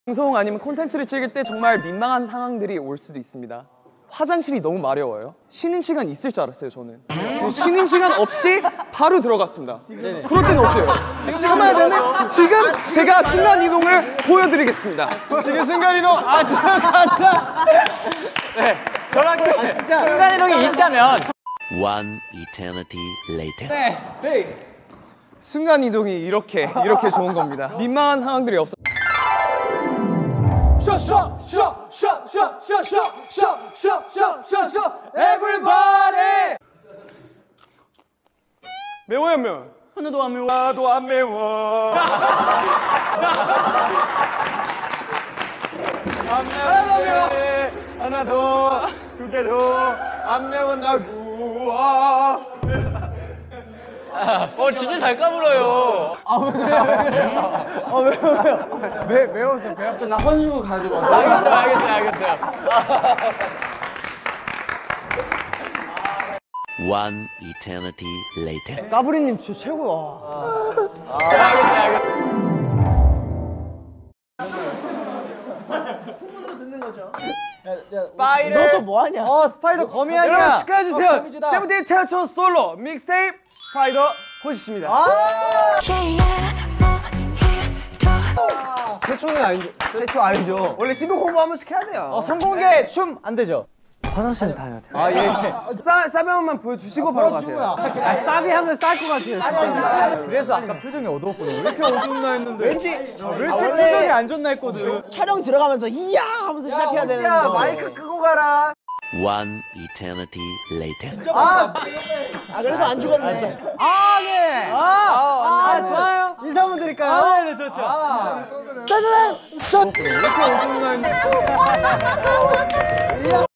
Seventeen members taking bathroom breaks on camera is so relatable 😂 (cc available)
Seventeen members Hoshi, The 8 and Joshua going to the bathroom while shooting is so relatable like kids asking for bathroom breaks in class. This video is dedicated to seventeen members being themselves.